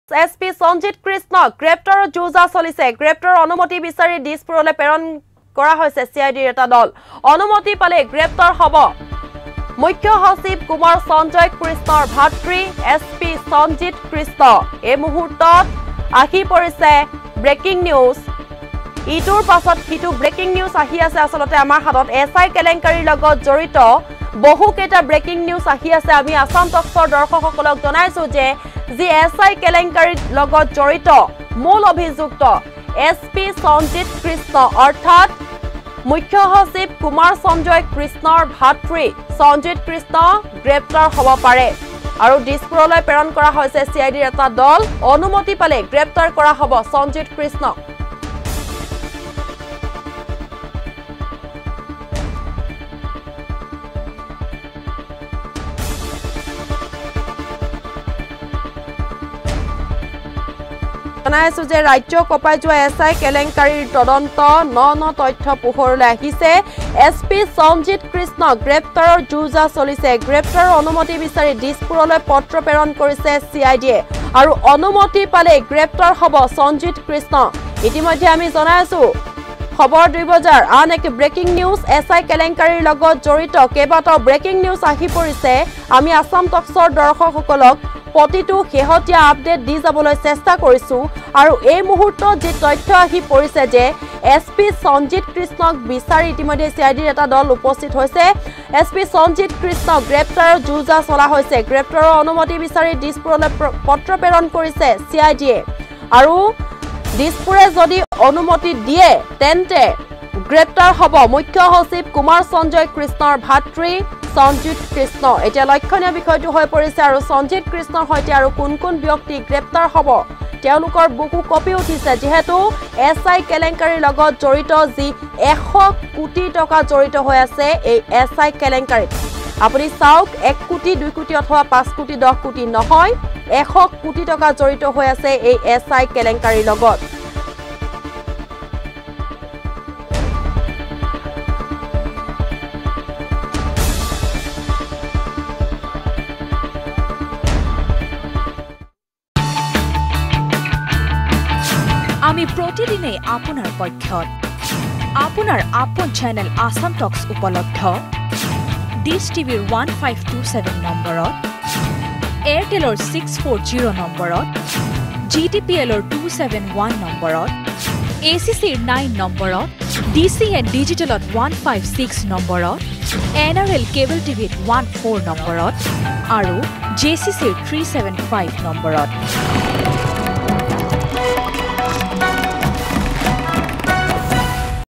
SP সঞ্জিত কৃষ্ণক গ্ৰেপ্তাৰৰ যো-জা। গ্ৰেপ্তাৰৰ অনুমতি বিচাৰি দিশপুৰলৈ পত্ৰ প্ৰেৰণ CIDৰ
SP সঞ্জিত কৃষ্ণক গ্ৰেপ্তাৰৰ যো-জা। গ্ৰেপ্তাৰৰ অনুমতি বিচাৰি দিশপুৰলৈ পত্ৰ প্ৰেৰণ CIDৰ। অনুমতি পালেই গ্ৰেপ্তাৰ হ’ব সঞ্জিত কৃষ্ণ।